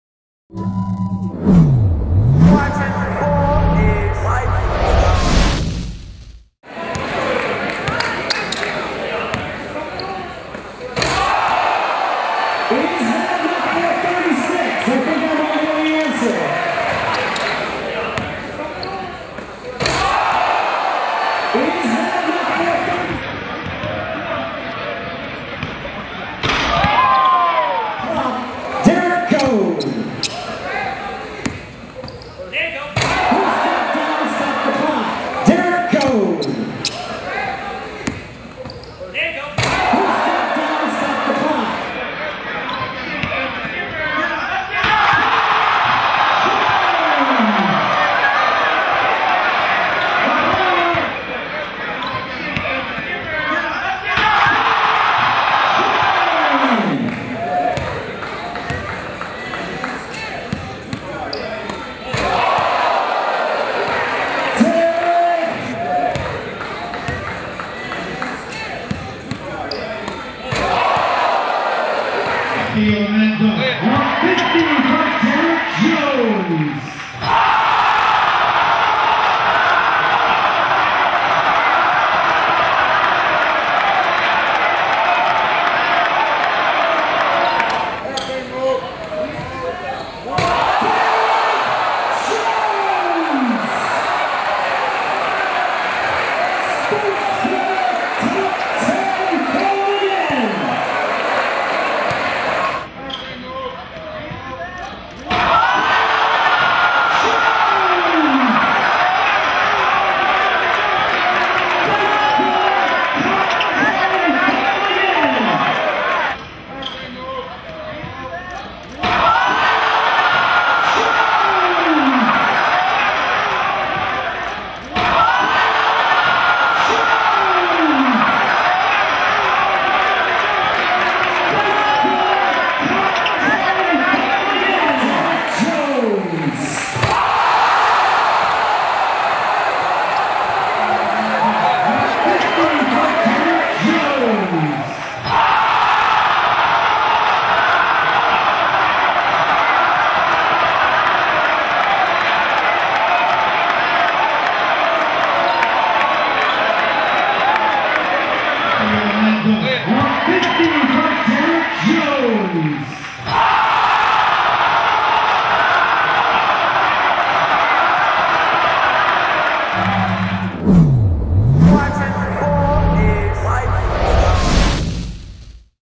Derrick Jones Upstages NBA Dunk Contest With INSANE Dunks at City of Palms Dunk Contest!
Derrick Jones is making a strong case for the best dunker not only in high school but in the nation period with his incredible highflying display at the 2014 City of Palms Dunk Contest! He single handedly upstaged any NBA dunk contest in recent memory.

Special thanks to Elite Mixtapes Youtube.com/elitemixtapes